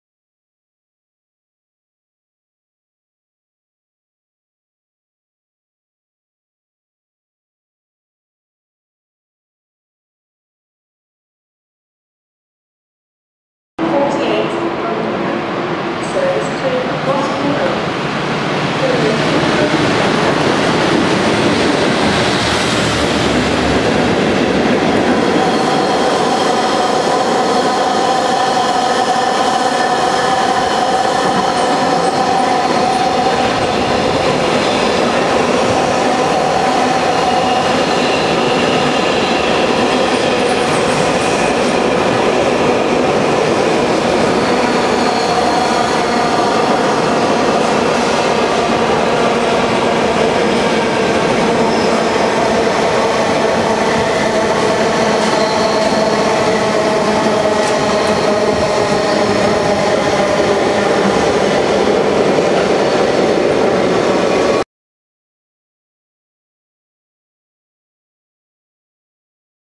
57304 & 37405 & 37612 on 4L48 Daventry Drs to Purfleet DRS Passing Barking Station 20/01/2014
DRS 57304 & 37405 & 37612 on 4L48 Daventry Drs (Malcolm W H) to Purfleet Deep Wtr Wf (Flt) Passing Barking Station 20/01/2014
57304 the Rugby Thunderbird loco was sent to rescue 4L48 after 37612 failed North of Tring on the WCML.
57304 then worked the 2 37s through to Purfleet via Ripple Lane